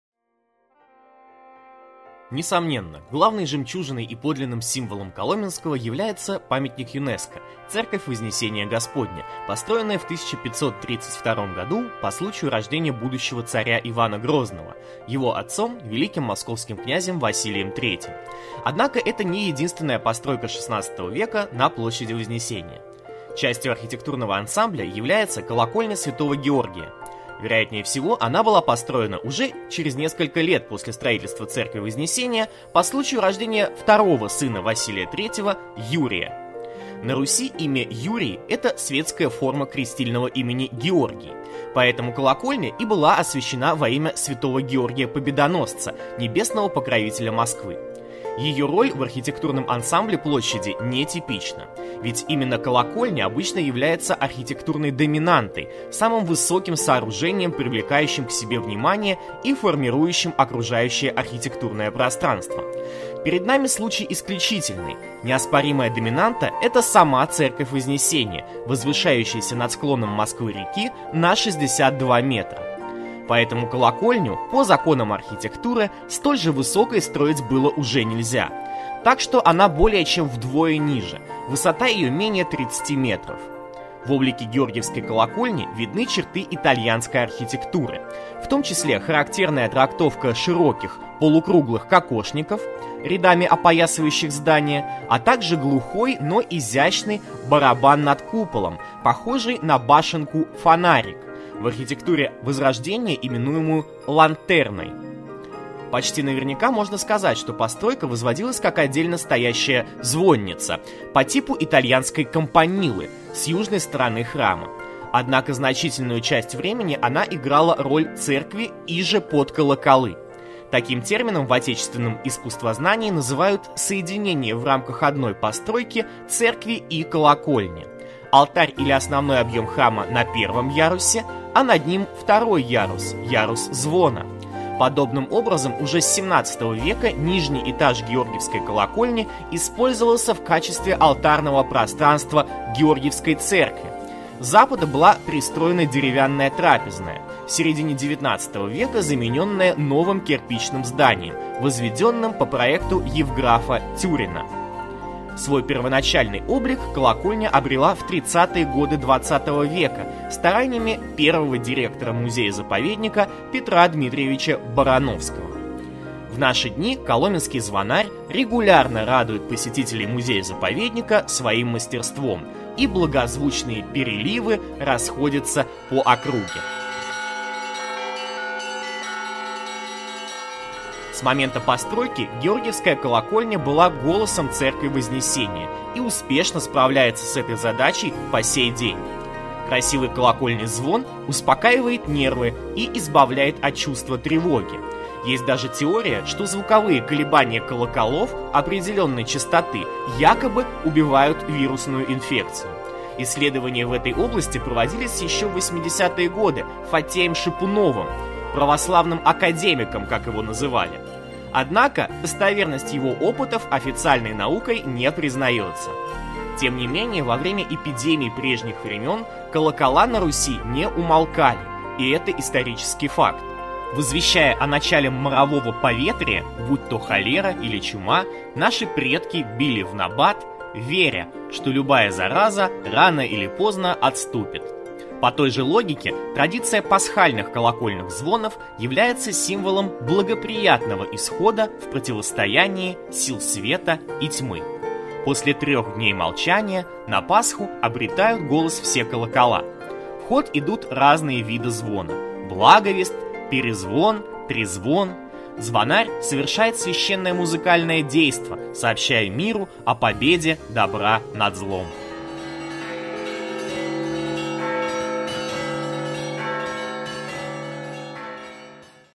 Ролик о Георгиевской колокольне и колокольных звонах
Историю о колокольне в честь Пасхальной недели снял сотрудник отдела экскурсий и экскурсионно-художественных программ музея-заповедника «Коломенское» Дмитрий Котов.